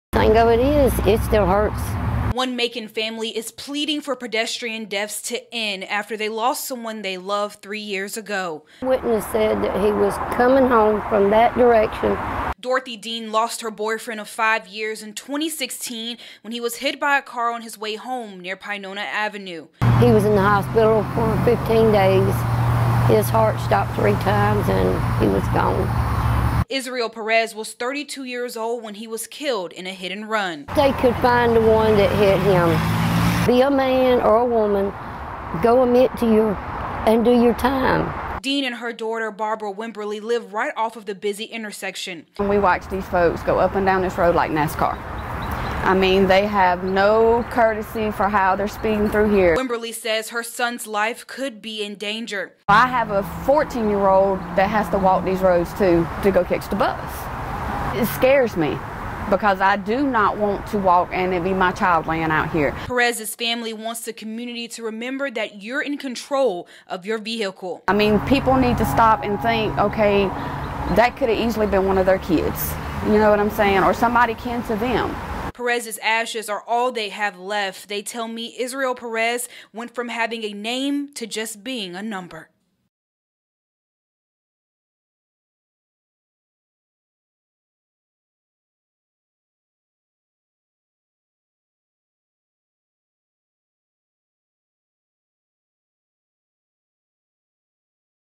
Victims' families hope Bibb leaders will make Macon safer for pedestrians
'I hope they find the one that hit him:' One Macon family is pleading for pedestrian deaths to end -- after they lost someone they loved three years ago in a hit and run